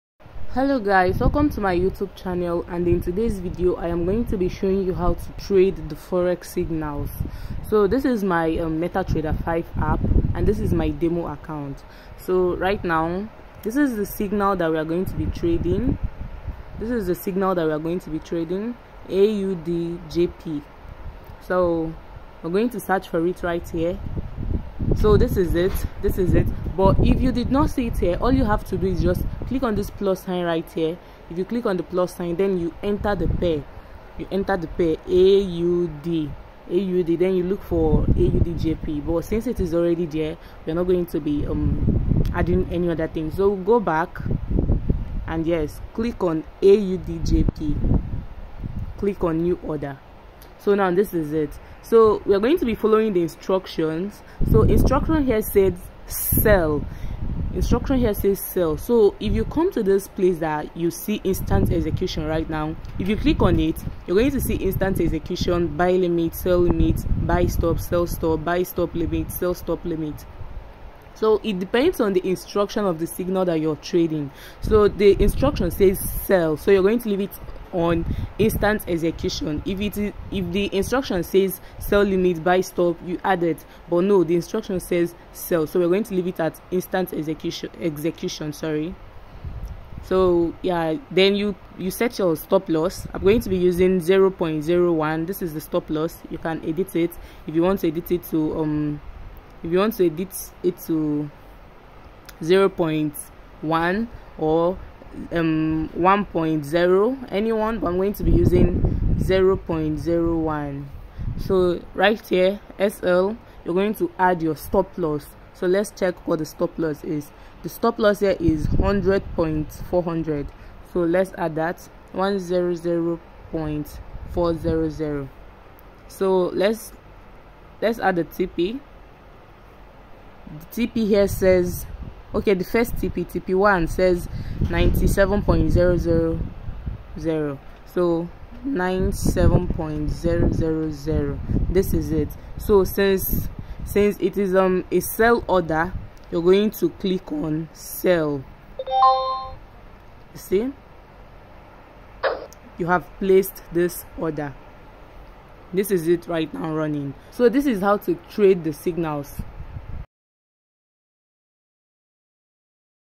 How To Copy And Paste Signals on Mt5 in less than 3 mins. Step by step tutorial for beginners
Learn how to copy and paste signals in less than 2 minutes!!!
Step by Step tutorial for beginners. Forex trading education

You don’t have an Exness account yet?  Click the link below to create an account ⬇️⬇️